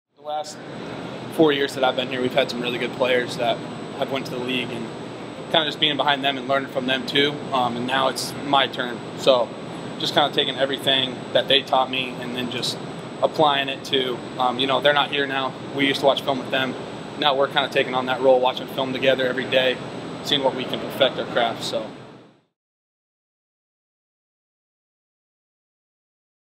Colton Feist: 'Now it's my turn'
Colton Feist (82), a Nebraska defensive lineman, speaks following a Husker football pre-season practice at Memorial Stadium in Lincoln on Monday, August 8, 2022.